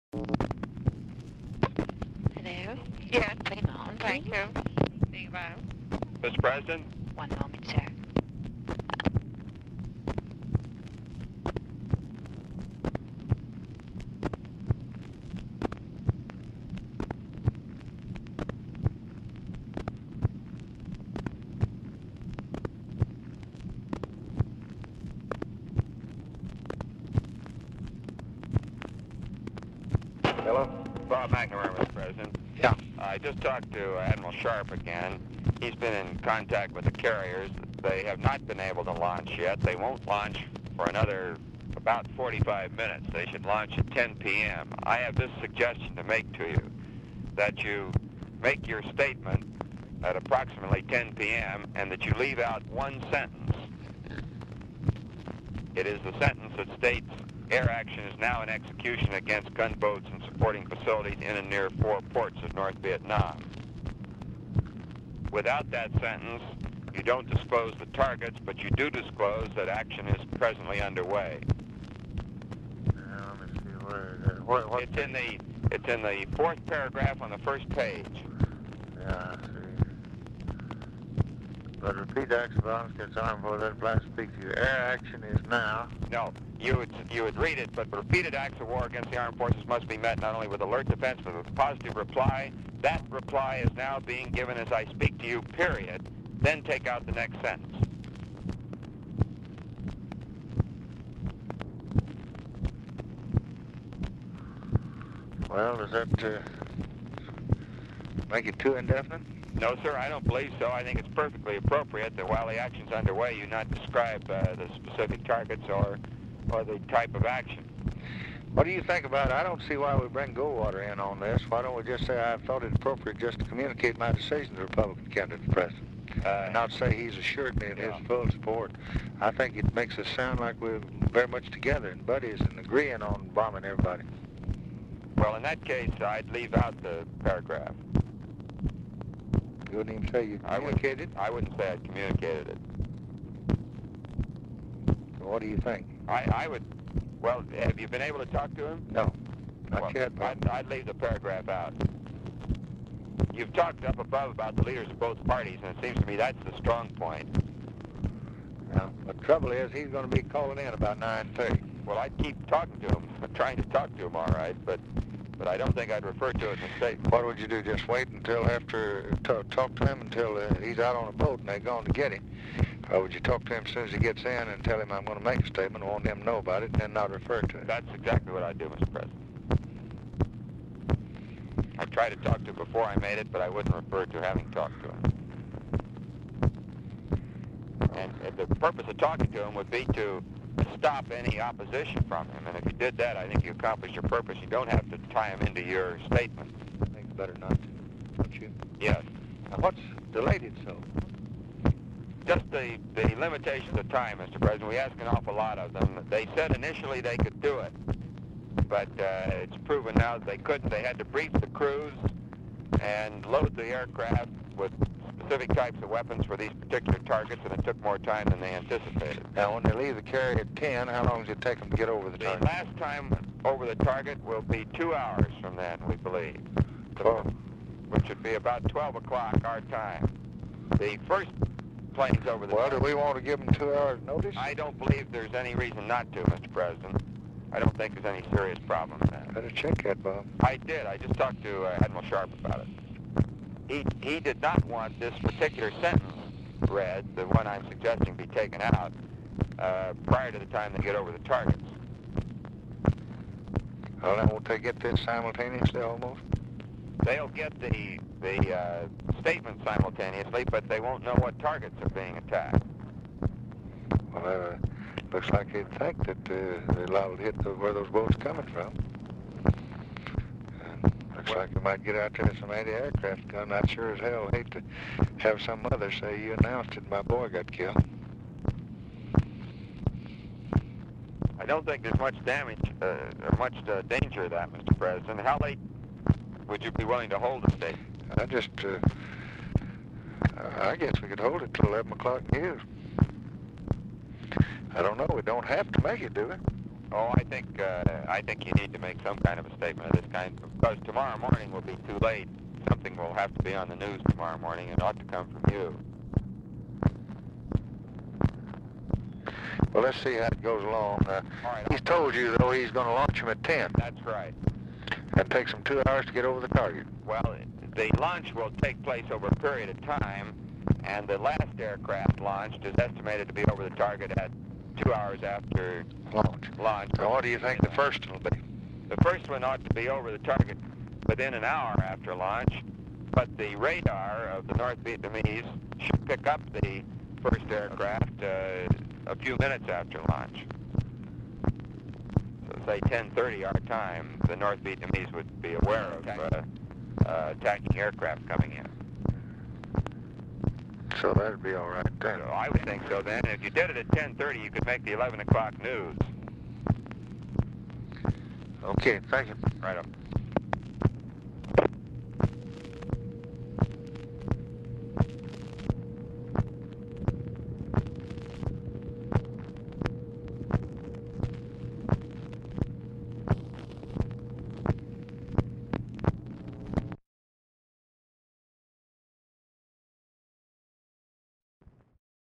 LBJ and Robert McNamara, 8/4/64, 9.15p.
Telephone Conversation between President Johnson and Robert McNamara.

Time: 9:15PM

Speakers: President Johnson and Robert McNamara.

General Topics: Defense; National Politics; Speeches; Vietnam

Topics: McNamara Reports On Delay Of Aircraft Launch In Retaliatory Raid Following Tonkin Gulf Attacks, Recommends Deletion Of Sentence On Targets From LBJ's Statement; Efforts To Reach Goldwater; LBJ Questions Whether Timing Of Address Will Endanger US Forces